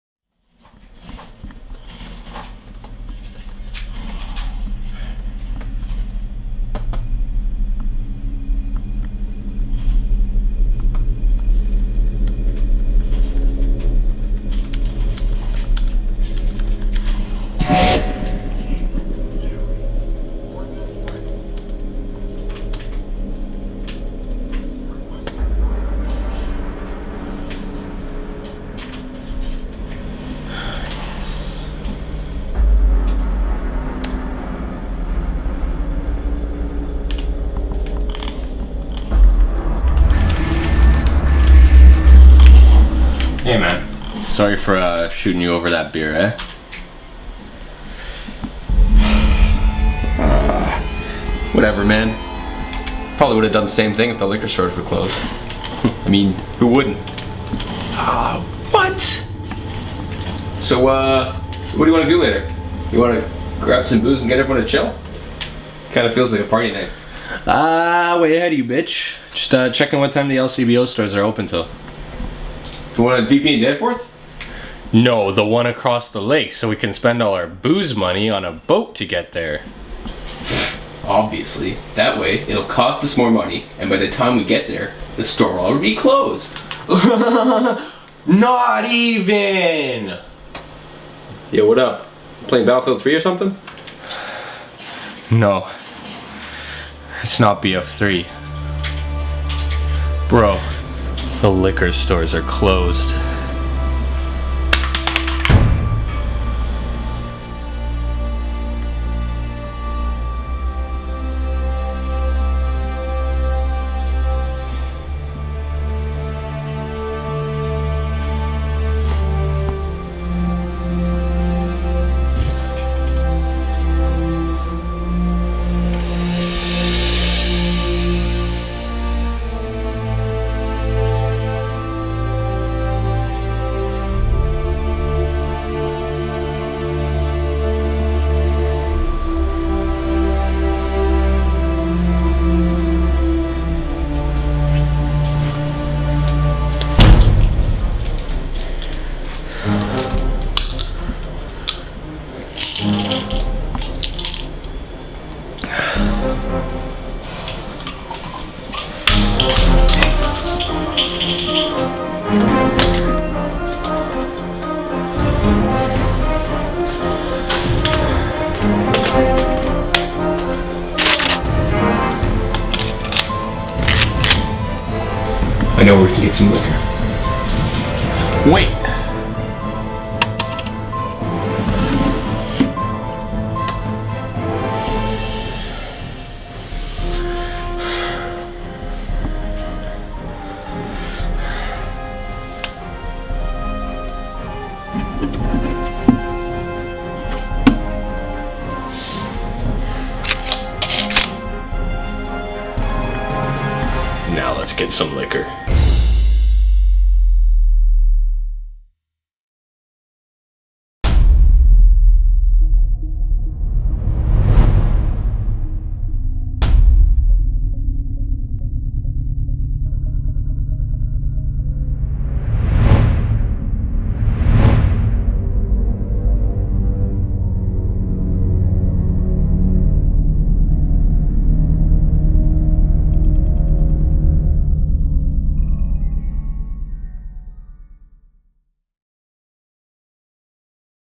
Old English Violence 2 (Part 1)
Maybe we're becoming addicts because what we do to get our liquor is a little extreme. Again all the liqour stores seem to be closed. How do I get my liquor this time! Another robbery perhaps? All I know is there's guns, violence and fighting, you gotta check this out!